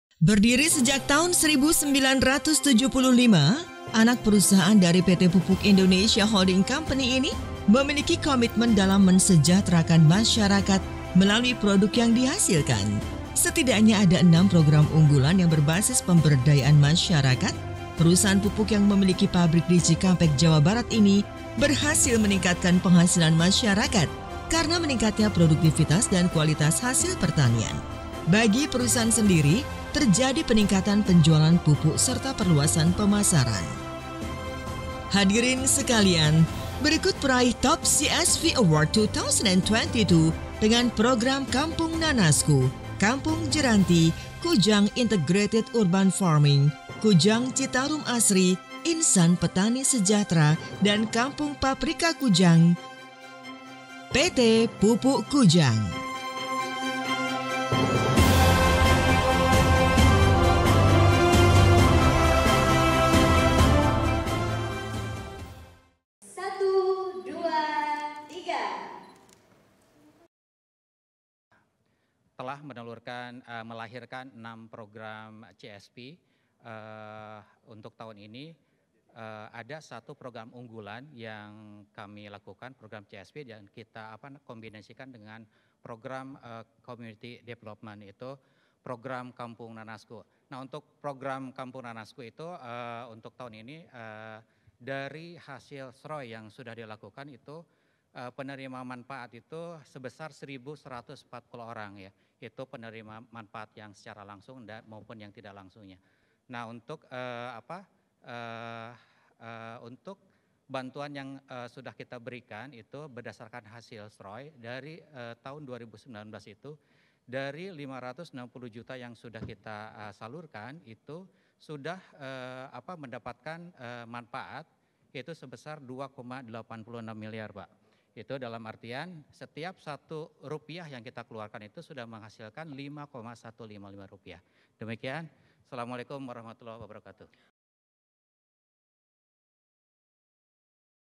PT Pupuk Kujang Sabet Penghargaan Top CSV Award 2022
Selamat dan sukses kepada PT Pupuk Kujang yang telah meraih penghargaan Top CSV Award 2022 .

Memiliki komitmen dalam menyejahterakan masyarakat melalui produk yang dihasilkan, dengan beragam program CSV yang dimiliki telah berhasil meningkatkan penjualan pupuk serta perluasan pemasaran bagi perusahaan PT Pupuk Kujang.